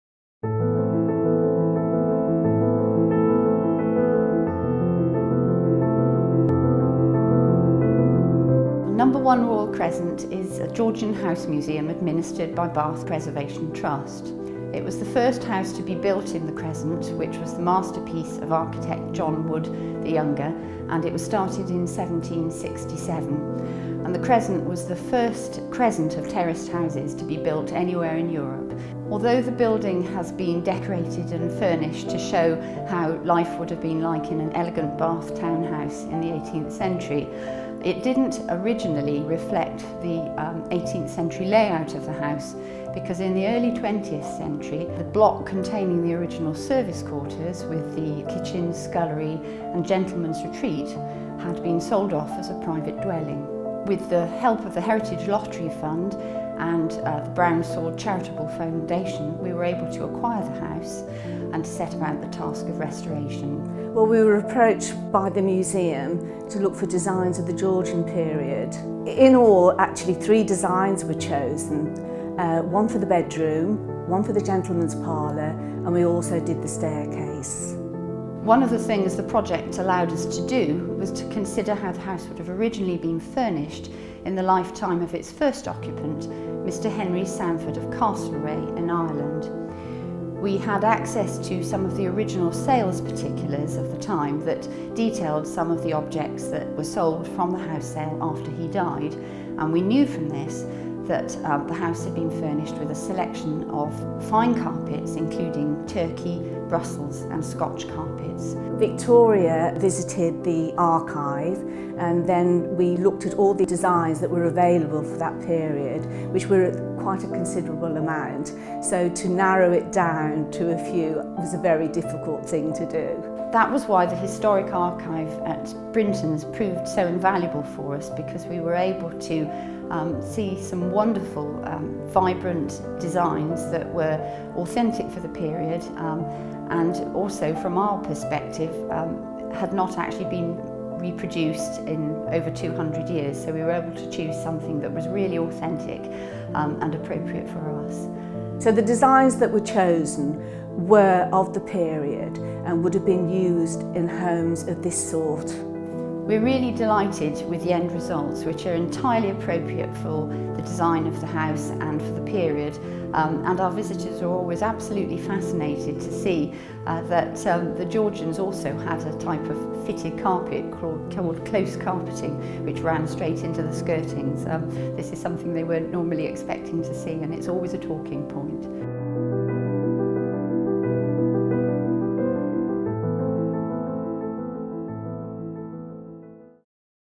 Brintons Case Study | No.1 Royal Crescent Museum, Bath
In June 2013, No.1 Royal Crescent was reunited with its original servants’ wing next door following a £5m renovation project. As a result the size of the museum has doubled, giving visitors a much richer insight into the life of residents in the late 18th century – both above and below stairs – whilst bringing the building up to modern standards.

Brintons Archivist, Yvonne Smith, worked closely with the curators of the museum to identify carpet designs which were authentic to the Georgian period. Designs were chosen from Brintons extensive archive which dates back to 1790. It is one of the world's largest commercial design archives and historical pattern libraries in the industry. Designs were chosen for The Parlour, Gentlemen's Retreat Lady's bedroom, Grand staircase and landing areas.